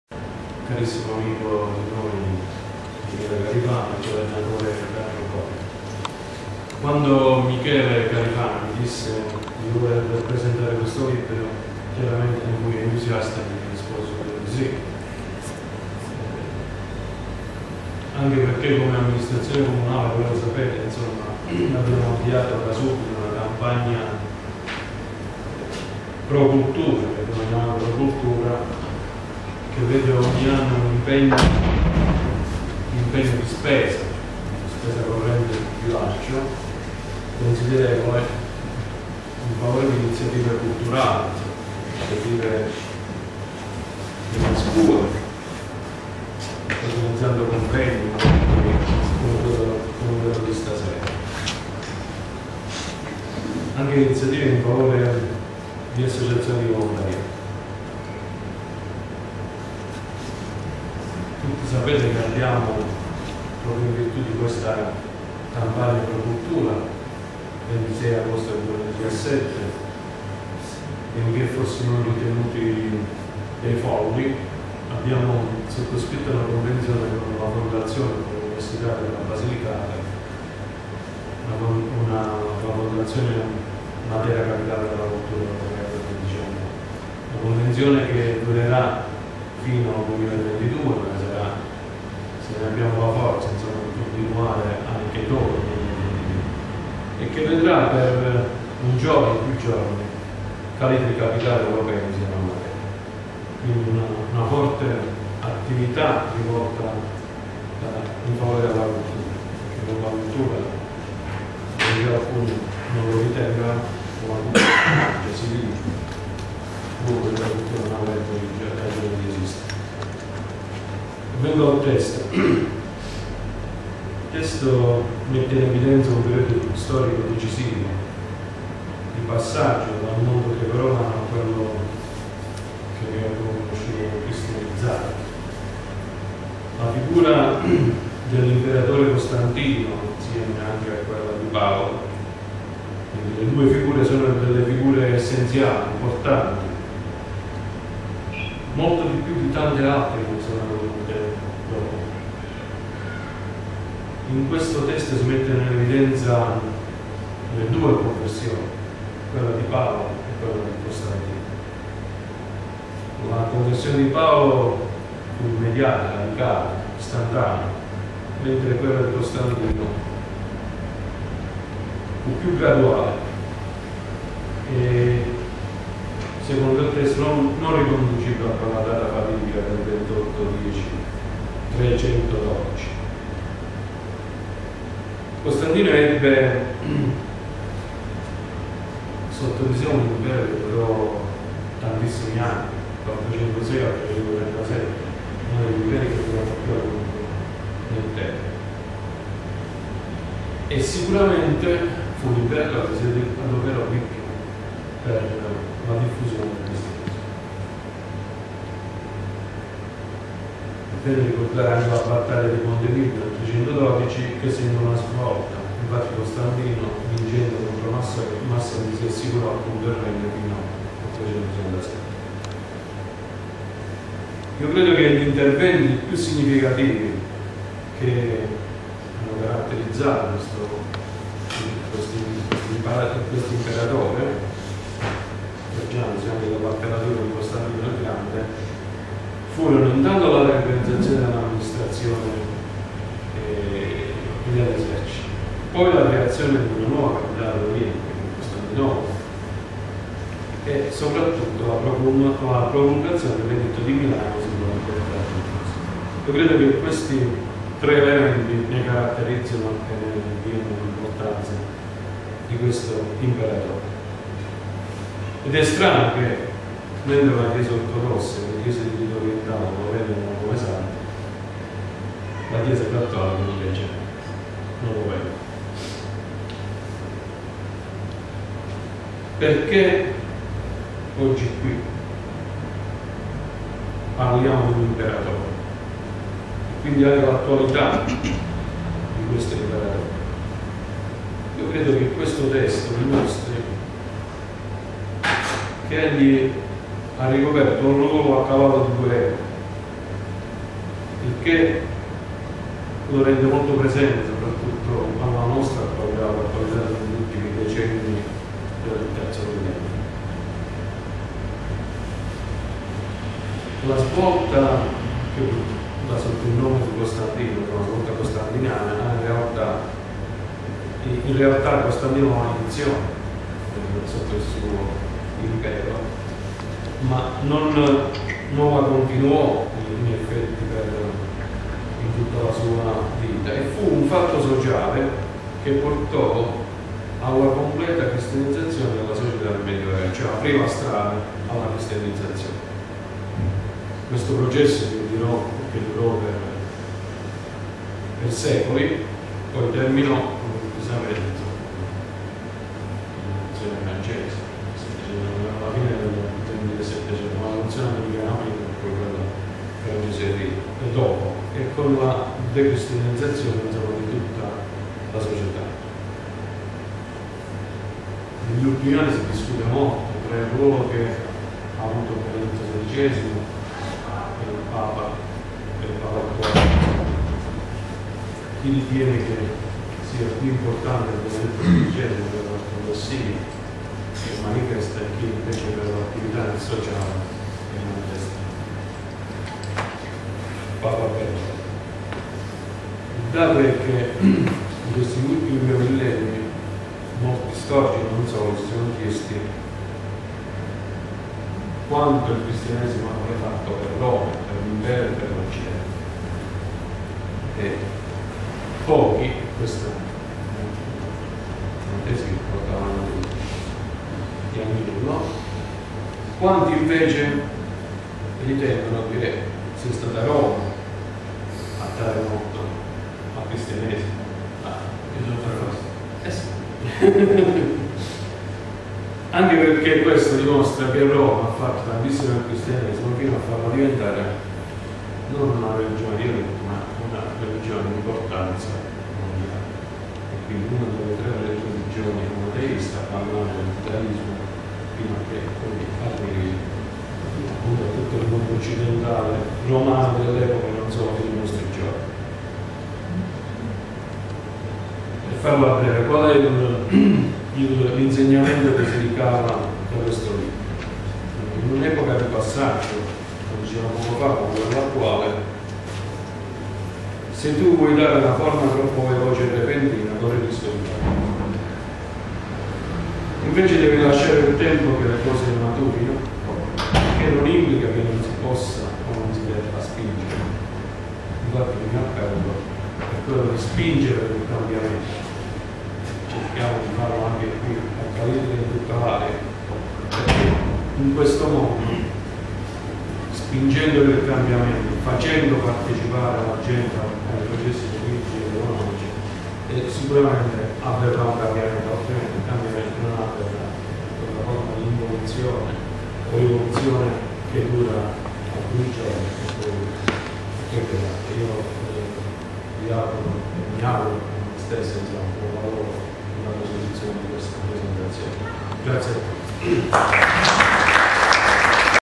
L'imperatore Costantino tra storia e leggenda - casa della musica di Calitri (Avellino)
Intervento del sindaco Michele Di Maio